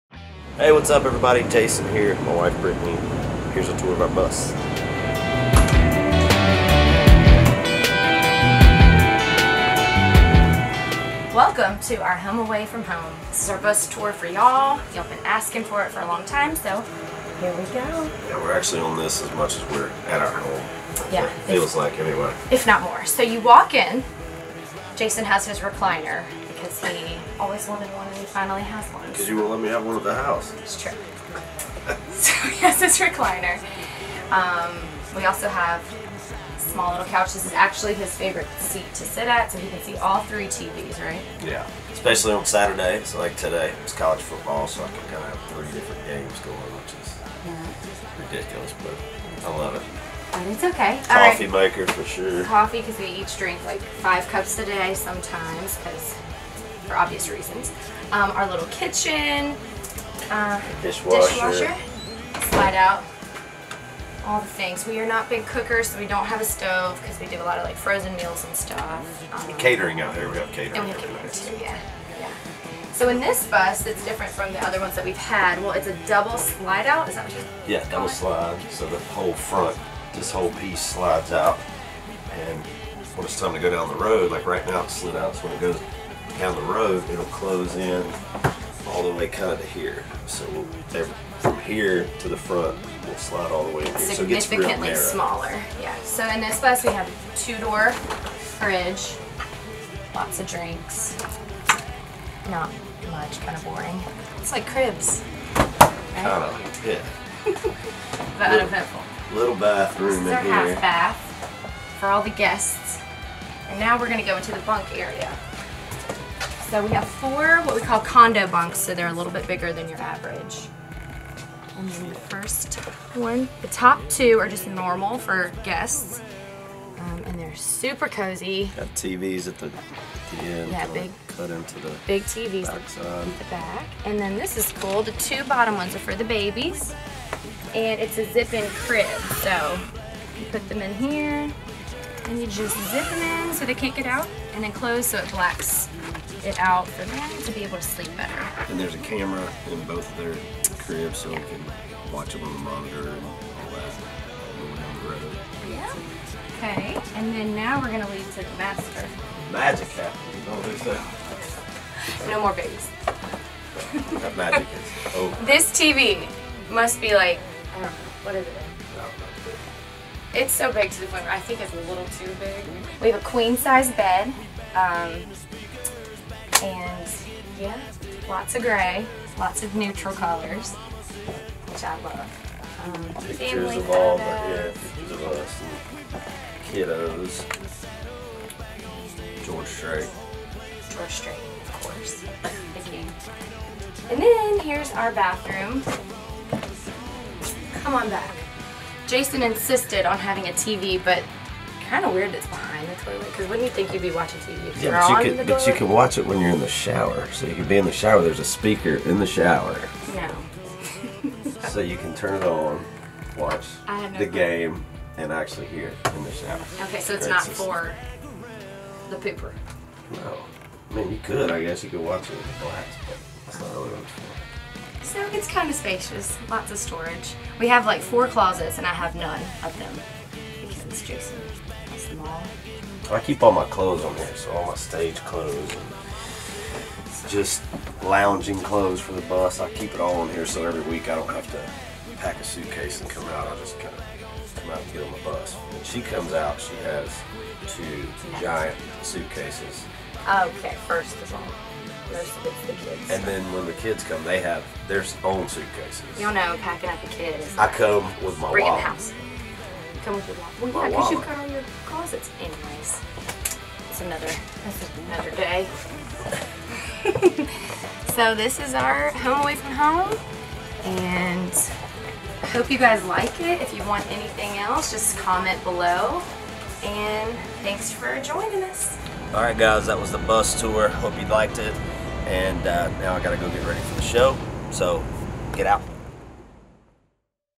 Jason Aldean - Bus Tour (2019)
Everyone is always asking about our bus so here ya go.

🎥: Justin Mrusek

To be the first to know about tour updates, sign up for the Jason Aldean email newsletter